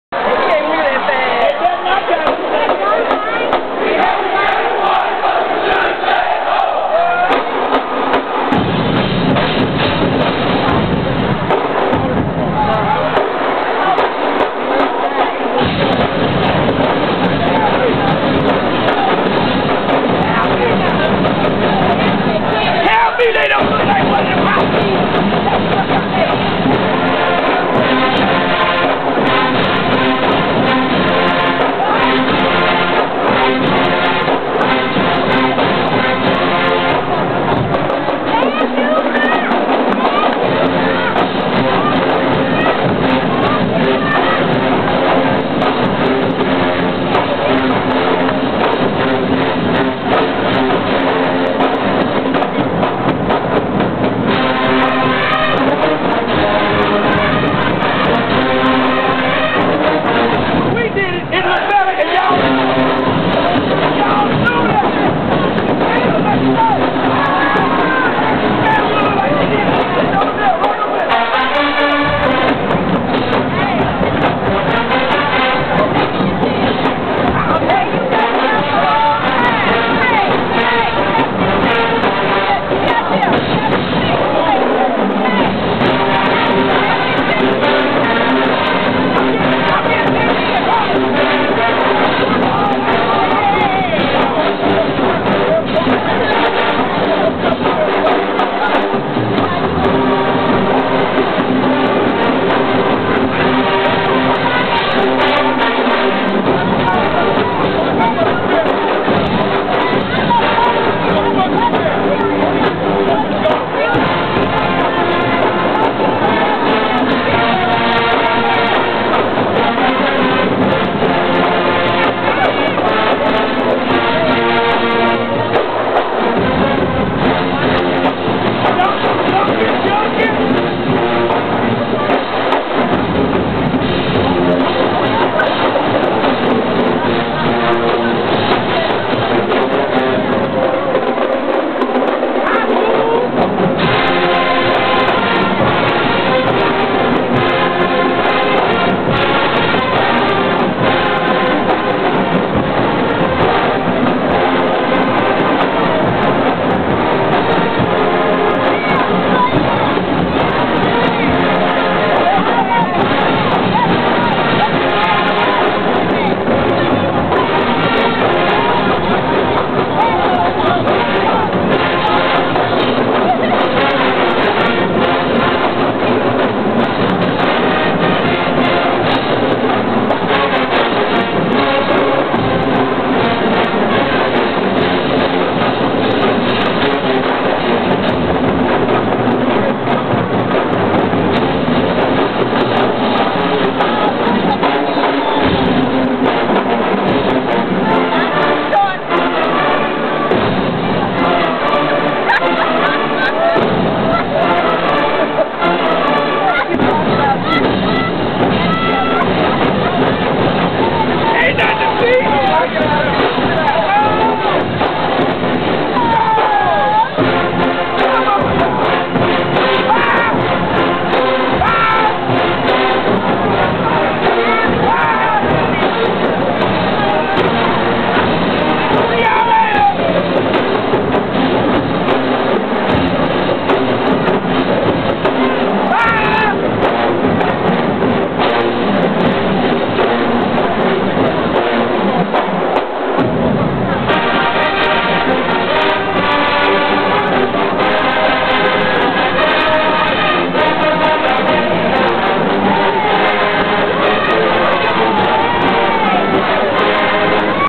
Dance Routine BOTB
The Marching 100 at the Florida Classic BOTB in 2006